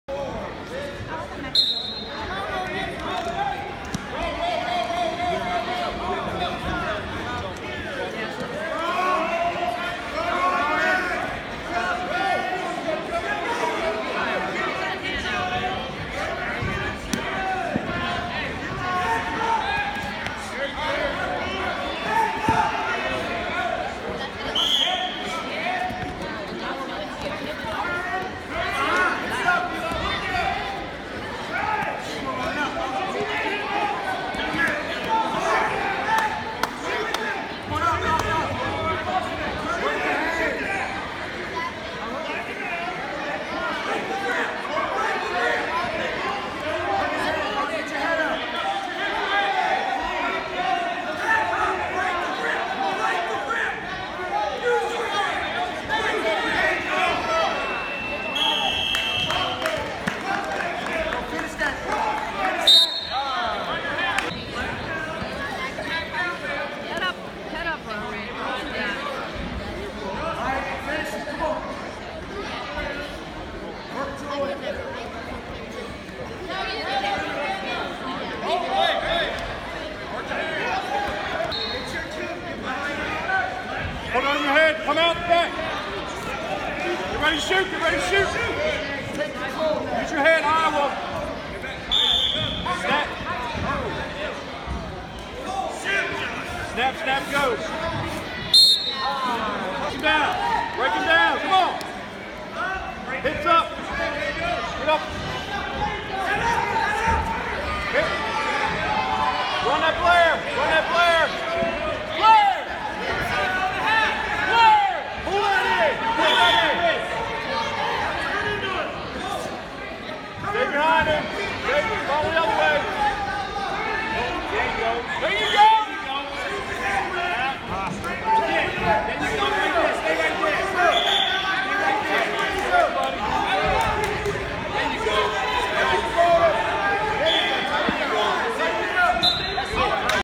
MMA Kid Rhonin Double Cradle Roll
MMA Kid Rhonin is training to be a UFC Fighter. UFC Kid Rhonin is a Wrestler, BJJ, Boxer and Muay Thai  Fighter. Rhonin "The Sherdog Kid" is on his way to greatness.